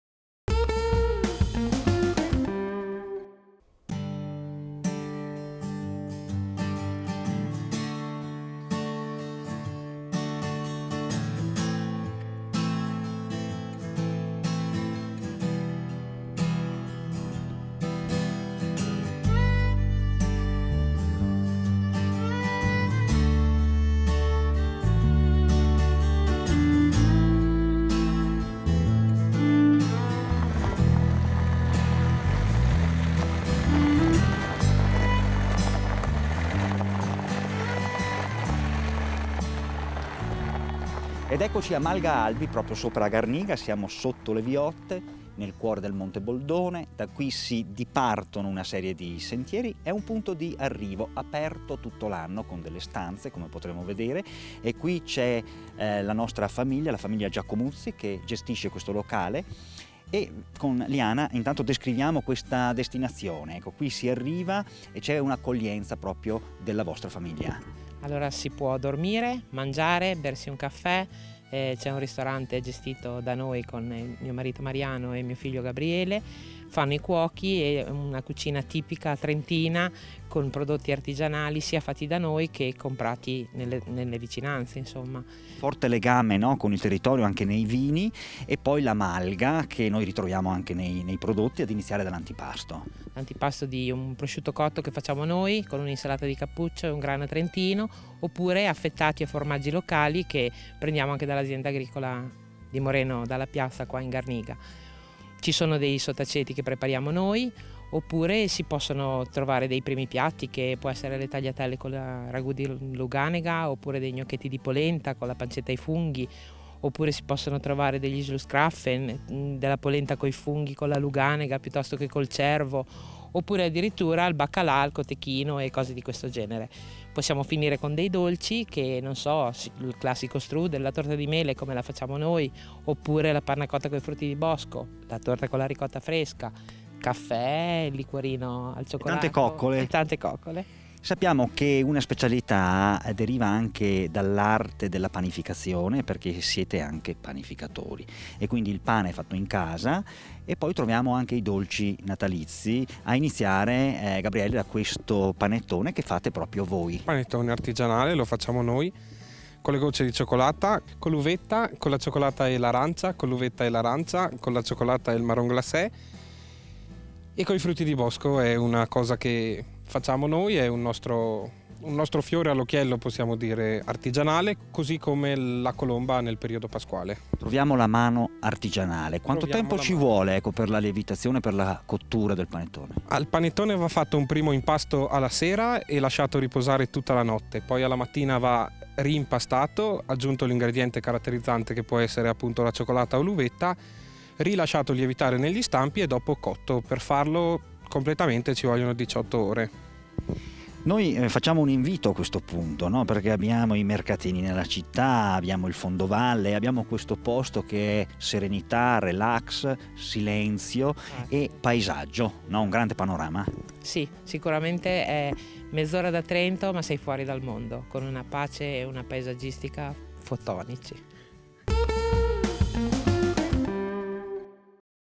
MALGA ALBI, I SAPORI DELL'ALPEGGIO A DUE PASSI TRENTO
Malga Albi è situata nella splendida conca omonima circondata da boschi di faggi, abeti e larici a 1263 metri d’altitudine nel comune trentino di Garniga Terme, noto per i suoi Bagni nell’erba curativi (curiosità: una leggenda vuole che il nome Garniga derivi dal tedesco Gar Nichts, assolutamente niente, in lingua tedesca, sarebbe ciò che dicevano i minatori riferito al fatto che qui non trovavano alcun minerale da estrarre). E’ stata costruita nel 1654 e ristrutturata nel 2006.

E’ una struttura accogliente con: 7 camere da letto, spaziose e piene di luce, tutte dotate di ampio bagno, per un totale di 20 posti letto, arredate in stile alpino con utilizzo di legname d’abete e pavimenti di larice, una sala ristorante ampia e luminosa dove gustare sapori e profumi dei prodotti agro pastorali di Garniga e del Trentino nei piatti proposti dai cuochi Mariano e Gabriele.

In menù salumi, formaggi e ricotte alle erbe dell’Azienda Agricola locale di Manuela e Moreno Dallapiazza; nonché le verdure dei campi vicini e le tisane aromatiche prodotte da Loris Cimonetti titolare dell’Azienda Agricola Biologica Ecoidea S.a.s..

Nella splendida conca dell’Albi si potranno vivere le proprie vacanze e trascorrere il tempo libero passeggiando nei dintorni, giocando – per i più piccoli, ma anche per chi vuole tornare bambino – con gli animali che trovano ospitalità nel “baito” sotto la malga. Per chi vuole farsi coccolare, vi è la possibilità di usufruire, con sconti personalizzati per i clienti della Malga, dei percorsi del benessere alle erbe del Monte Bondone e dei trattamenti benefici, rilassanti ed estetici alle Terme di Garniga purtroppo momentaneamente chiuse.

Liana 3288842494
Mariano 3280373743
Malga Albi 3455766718
Malga Albi si raggiunge percorrendo la Sp 25 che dal paese di Aldeno sale per il Monte Bondone. Arrivati a Garniga Terme prima di entrare in paese, si svolta a sinistra per la frazione Gatter, da lì in dieci minuti d’automobile si arriva a destinazione, , strada asfaltata fino a 500m. dalla Malga.